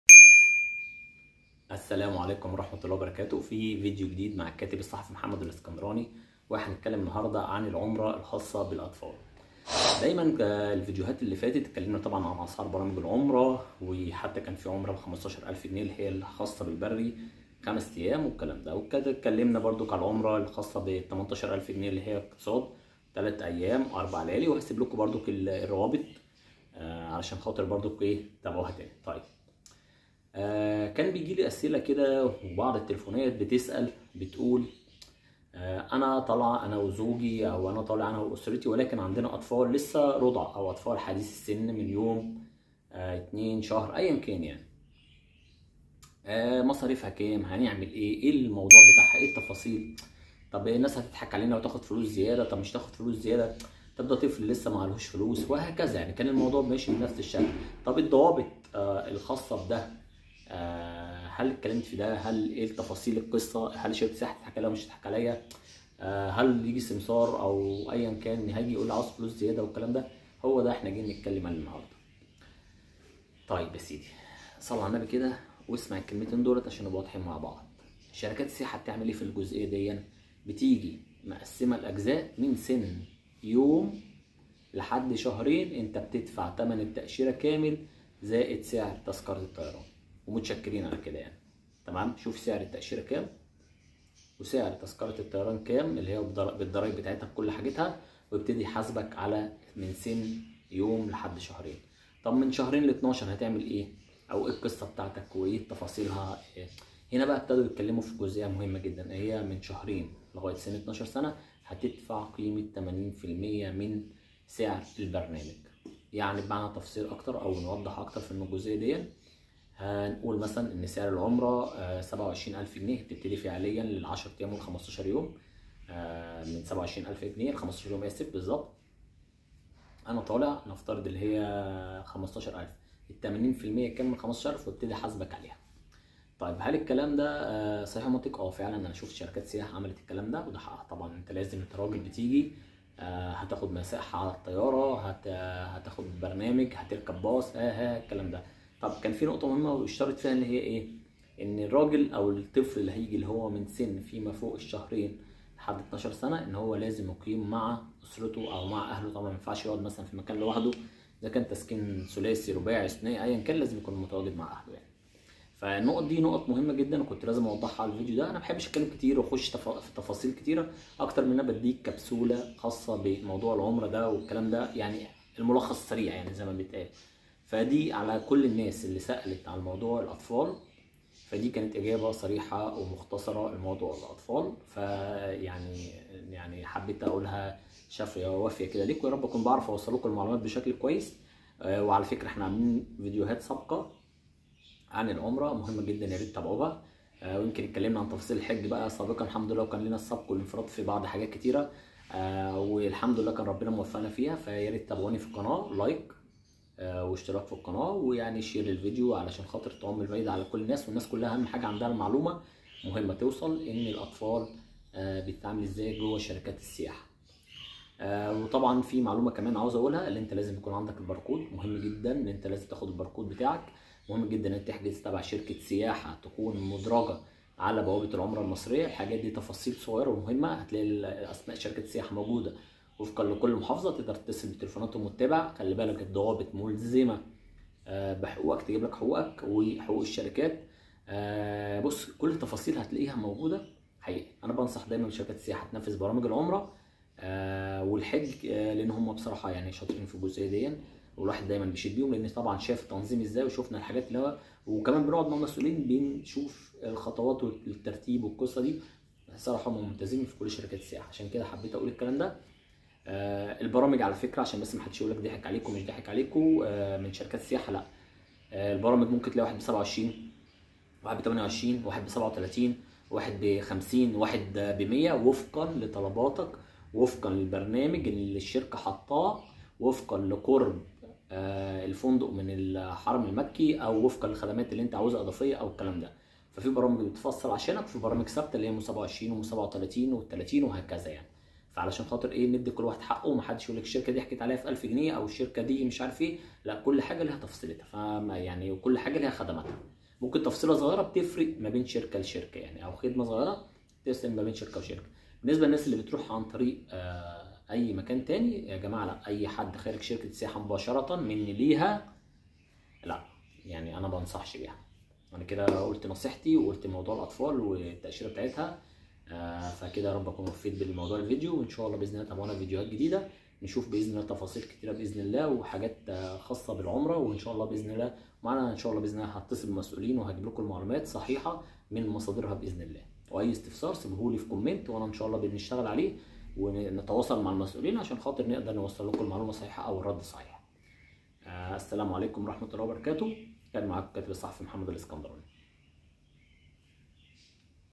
أسعار العمرة للأطفال .. وأسعار العمرة 2023
أسعار العمرة الآن للأطفال وأسعار العمرة 2023 أسئلة جائتني من المتابعين الحريصين على أداء العمرة من مصر التي تعد ذات أهمية قصوي لجميع المصريين لأداء مناسك العمرة 2023.
سعر العمرة للأطفال كثير من يبحث عنه في ظل السفر لأداء مناسك العمرة 2023 ، بصحبة أطفالنا وهو ماسيتم الإجابة عليه عبر الفيديو مع الكاتب الصحفي محمد الاسكندراني.
شروط العمرة للأطفال ، هي نفس شروط الكبار واختلاف السن ولكن تختلف في قيمة السعر المعلن للبرامج الخاصة بـ عمرة 2023 .
سعر العمره للأطفال وفقا لاختيار المعتمرين ، خاصة ان العمرة والمعتمر مرتبطان معا عبر برنامج تقدمة شركات السياحة في مصر عبر برامج عمرة متنوعة مابين البري و الطيران.
أسعار العمرة الآن أصبحت متاح لدي جميع شركات السياحة المصرية و التي تبداء من أسعار تقدمها وفقا لكل برنامج على حسب ماتعرضة على عملائها
أسعار العمرة 2023 بدأت تترواح مابين 15 الف جنيه وفقا للبرنامج البري 5 أيام مرورا الى بداية من 27 الف جنيه طيران اقتصادي وارتفاعا للسياحي.
سعر العمرة للمصريين يتسائل عنه الكثير من المعتمرين الا انه يختلف وفقا للبرنامج الذى يريده كل معتمر